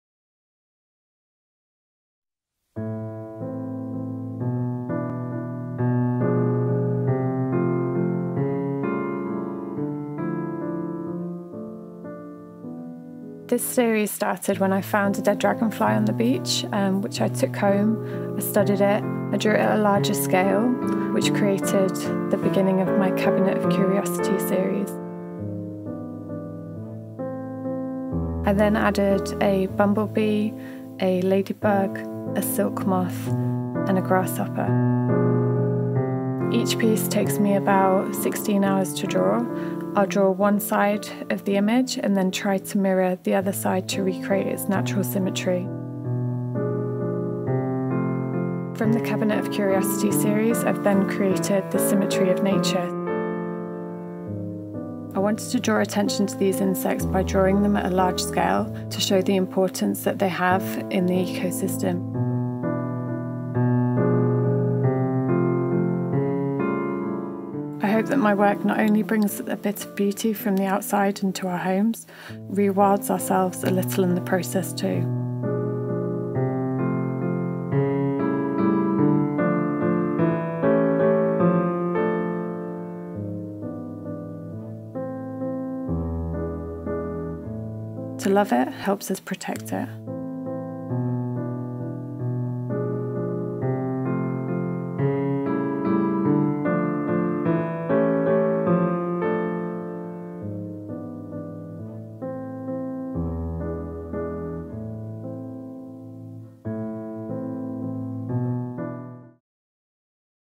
Kube Final 1080P HD
Sarah Hammond presents ‘Symmetry of Nature’, her solo art show at the Kube in Gibson, featuring her 'Cabinet of Curiosity' series. The pieces displayed focus on the entomology native to the Pacific Northwest, featuring large scale drawings of a dragonfly, a bumblebee, a ladybug, a silk moth, a butterfly and a grasshopper.

Sarah is a visual artist known for her fine line and highly detailed pencil drawings on paper. The level of detail she achieves is akin to a scientific rendering, but her approach is more fine art. Each cabinet of curiosity insect art work can take up to 16 hours to complete. The coloured pieces in the collection are created using the same techniques before having a little photoshop magic infused into them. They are then reproduced as high quality Giclee prints and various smaller digital prints.

Inspiration for ‘Symmetry of Nature’ presented itself when Sarah, out walking on the beach one day, found a dead dragonfly and decided to take the insect home to study it more closely. First, she photographed it, before painstakingly drawing the dragonfly on a blown up scale to really capture its exquisite detail. To recreate the fragility of the wings she concentrated intently on one side before mirroring the other with a directed emphasis on its natural symmetry. Symmetry in nature, and by extension human perception, has come to be known as a reliable metric of quality and beauty.

By representing such small creatures as larger iterations, Sarah is drawing attention not only to their beauty but also to their integral contribution to the ecosystem. Many animals rely on pollinators to create and maintain such habitats, but by virtue of their size, these insects can often be overlooked. Through their amplification Sarah aims to make such insects more visible; a metaphor that highlights the plight of these delicate microcosms, and one that throws our own vital but often neglected relationship with them into stark relief, too.

Sarah has always been a passionate studier of the natural world, an interest that has forayed into taxidermy. Unwaveringly drawn to National History museums, with London’s being her favourite, Sarah deeply aligns with their message of education, conservation and of invoking reverence for the natural world. After all, imitation by its very nature is an act of creation, and through her art Sarah seeks to honour all creatures, not only great and small, but also small and great.

Exhibition at The Kube June 2020
104-875 Gibsons Way
Gibsons, BC

5% of all sales will go to CPAWS, a charity that protects the parks and wilderness of Canada. Sarah is most inspired and impassioned by the beauty of the natural world and as such by the pledge of its conservation.